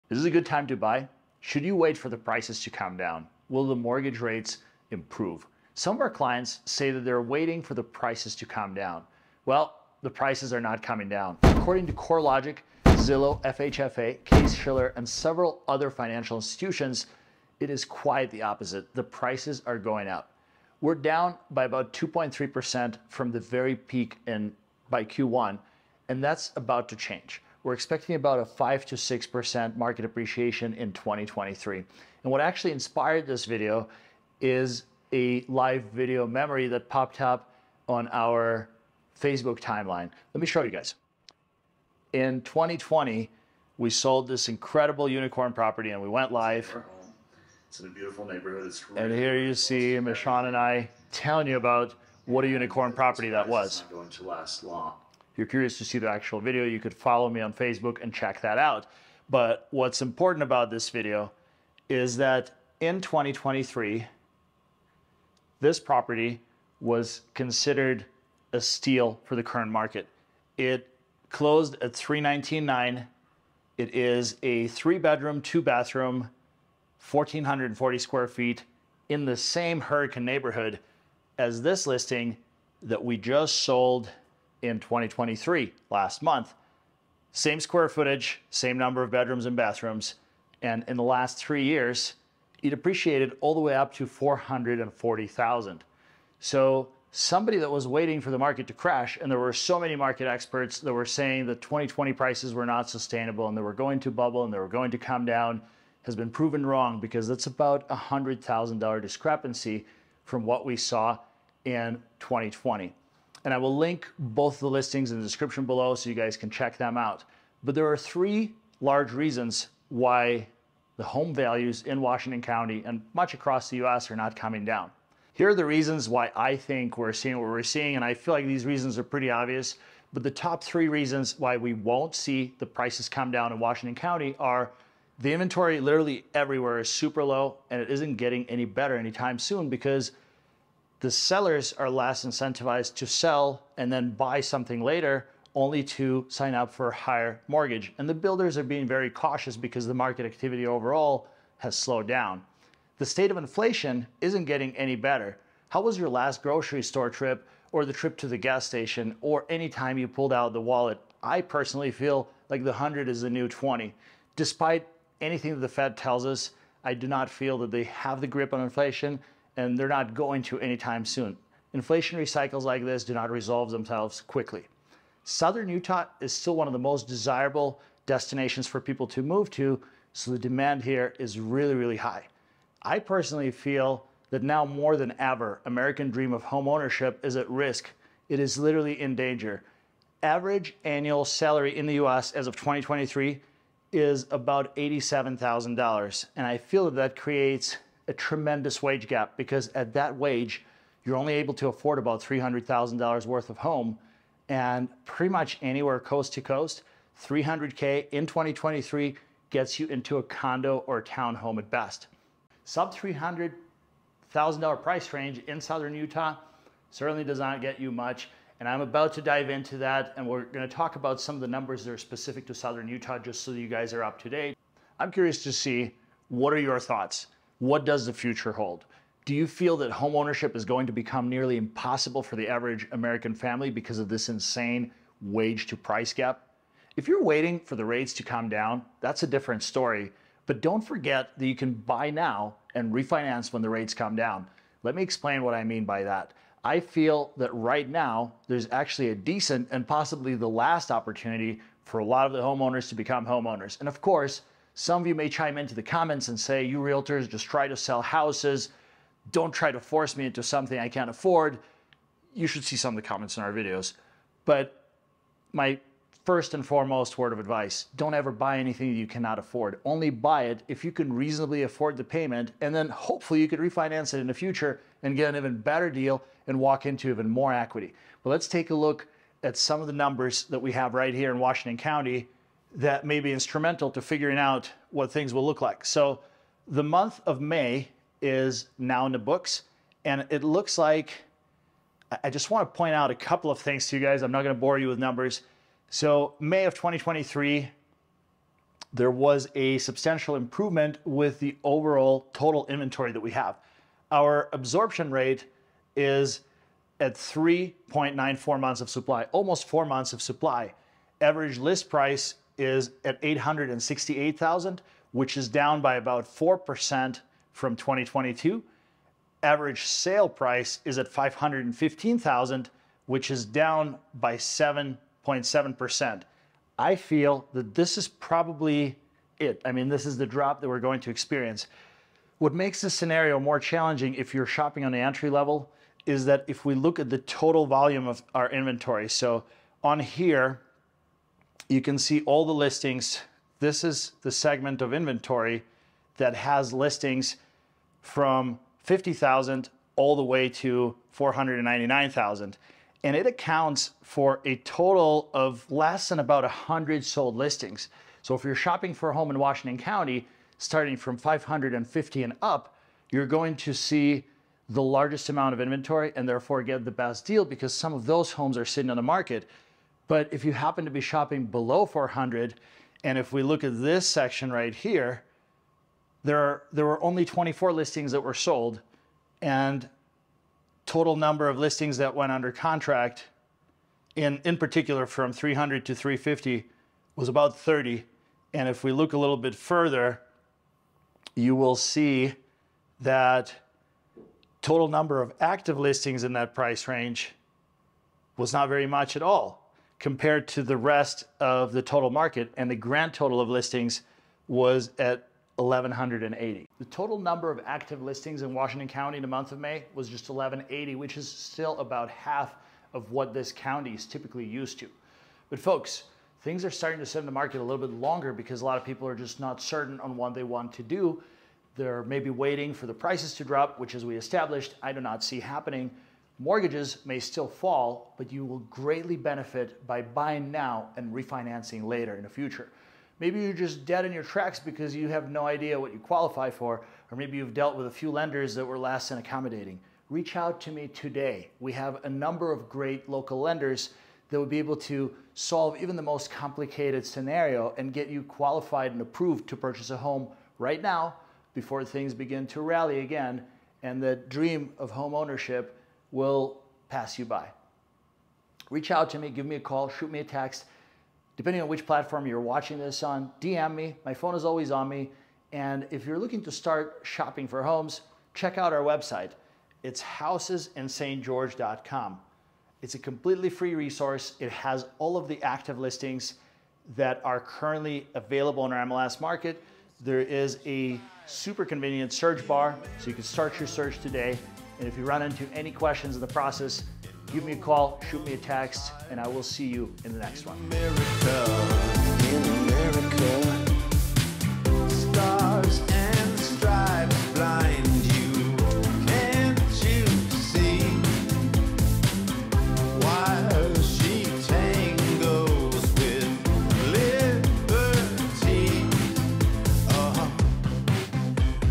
st george home prices are not coming down
st george home prices are not coming down period! In this video we will discuss an interesting case study of home value appreciation in Southern Utah in a community just minutes away from St George Utah.

We feel that the American Dream of home ownership may be in serious trouble. There are three reasons why the home prices in St George are not coming down any time soon if ever.

Video Chapters:
00:00 Introduction
00:24 St George Home values are rising
00:40 Over $120k in appreciation
02:10 Top 3 reasons why St George home prices are not falling
02:25 Low housing inventory
02:36 What inflation is doing to home prices
02:58 Huge demand for housing in St George Utah
03:11 American Dream of home ownership is at risk
03:41 St George Utah Housing stats
07:21 don't wait to buy a home in St George
07:53

🎦 Moving to St George - All things you should know before moving to Southern Utah
🎦 How’s the market? St George real estate market updates